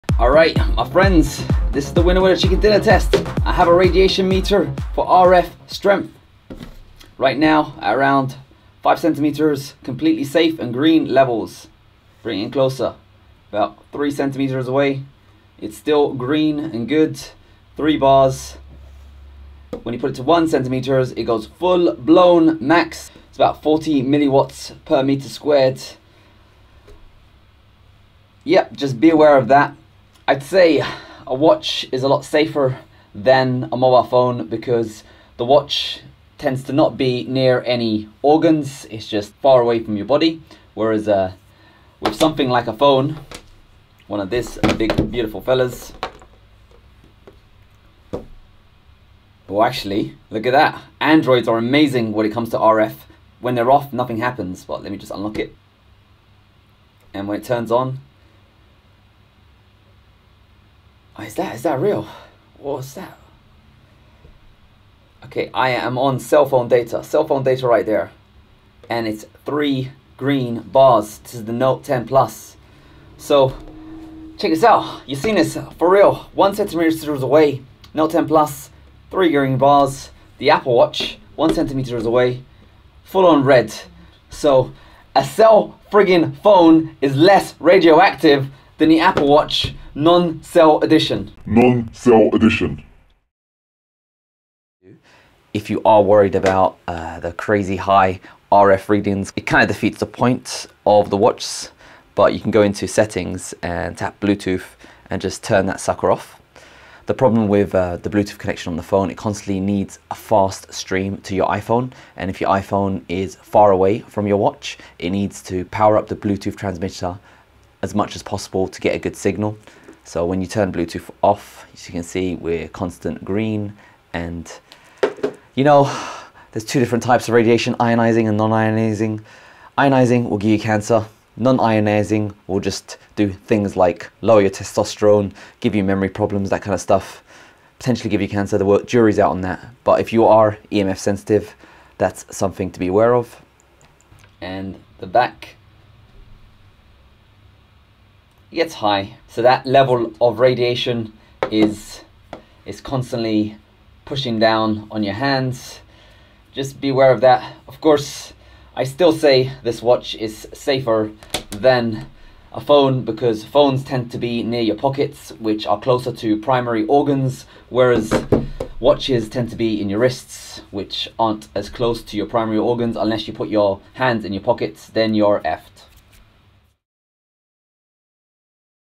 Apple Watch Series 5 has HIGHER Radiation than a Phone | RF Investigation
Apple Watch is THE ultimate tool for consumer health, but how safe is it? In this report we test how potent it is for non-ionizing radiation.

NON-IONIZING RADIATION
Non-ionizing radiation has been scientifically linked to the reduction of testosterone, memory loss, learning ability and even cancer in rats[1][2]. To learn more about the risks of cell phone radiation, I recommend an excellent talk[3] from Dr Devra Davis[4] (Served on the US Presidential Chemical Safety and Hazard Investigation Board)

SPECIAL THANKS
Thanks for your viewing support and if you have any suggestions or would like to help us produce more videos, please